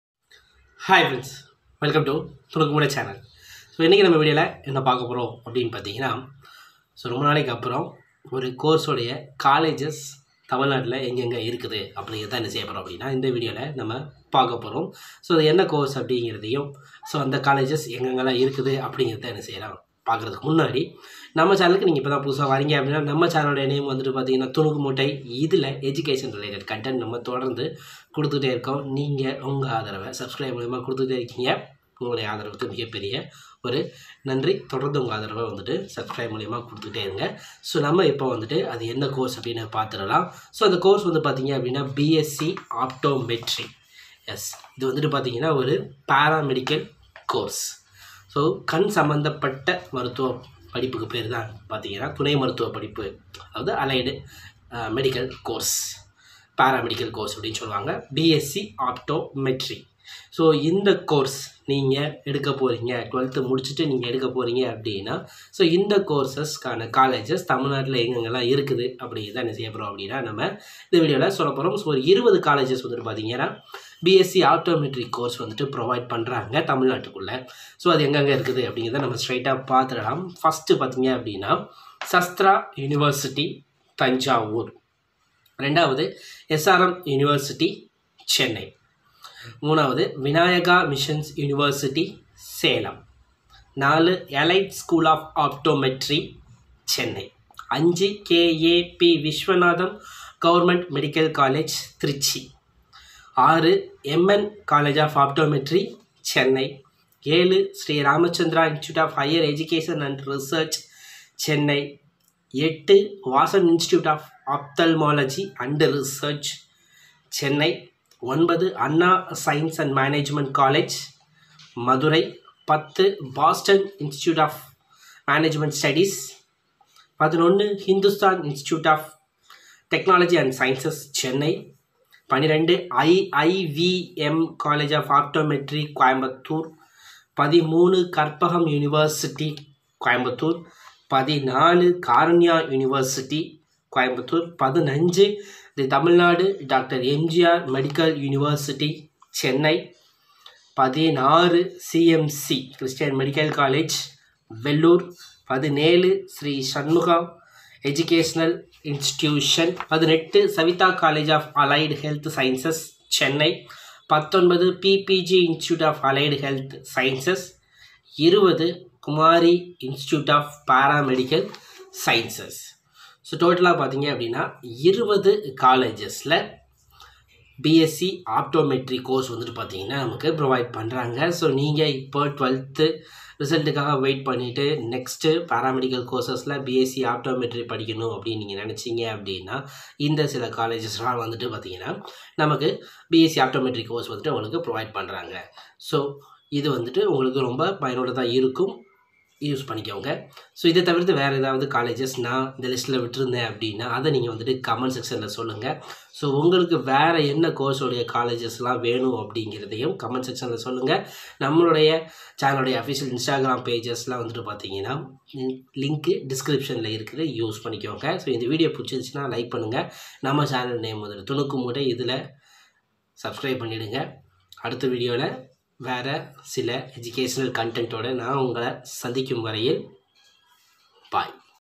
Bsc Optometry Colleges in tamilnadu| Paramedical college
This video gives you details about the Bachelor Optometry Colleges in tamilnadu. This is very useful to the students who search for the best colleges in tamilnadu for their studies.